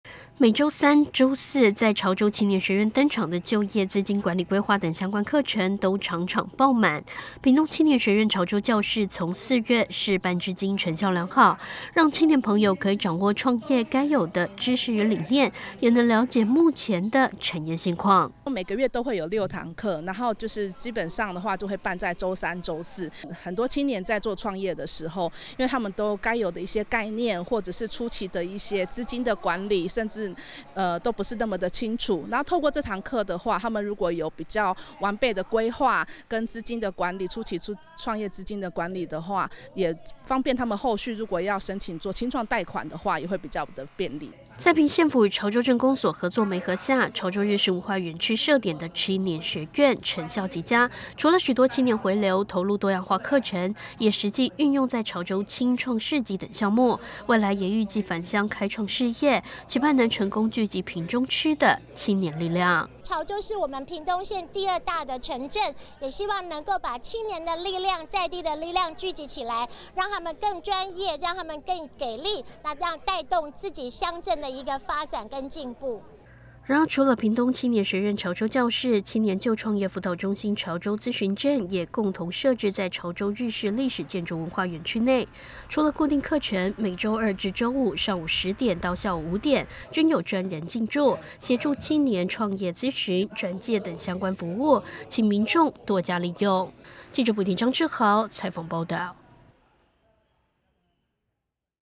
潮州青年學院試辦成效佳 潮青回流投入創業
屏東縣政府與潮州鎮公所合作在潮州日式歷史建築文化園區內，設點潮州青年學院，希望促進青年返鄉創業並擴大服務區域，如今4月試辦至今成效良好，每週開課報名都額滿，未來縣府將持續新增課程內容，鼓勵青年力回流。